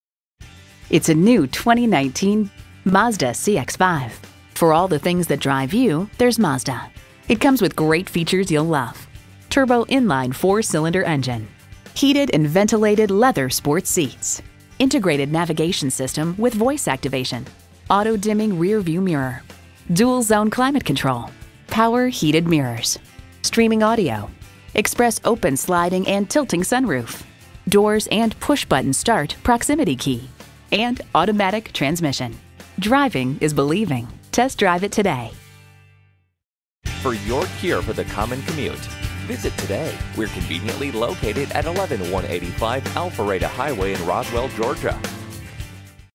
New 2019 Mazda CX-5 Roswell GA Atlanta, GA #532209 - SOLD
Year: 2019
Make: Mazda
Model: CX-5
Trim: Grand Touring Reserve
Engine: 2.5L 4-Cylinder Turbocharged
Transmission: 6-Speed Automatic
Color: White
Interior: Parchment
Stock #: 532209
VIN: JM3KFBDY3K0532209
Price includes: $500 - Customer Cash. Exp. 02/28/2019 Recent Arrival! Snowflake White Pearl 2019 Mazda CX-5 Grand Touring Reserve AWD 6-Speed Automatic 2.5L 4-Cylinder TurbochargedbrABS brakes, Alloy wheels, Electronic Stability Control, Emergency communication system, Front dual zone A/C, Heated door mirrors, Heated front seats, Heated rear seats, Illuminated entry, Leather Upholstery, Low tire pressure warning, Navigation System, Power Liftgate, Power moonroof, Remote keyless entry, Traction control.

Address:
11185 Alpharetta Hwy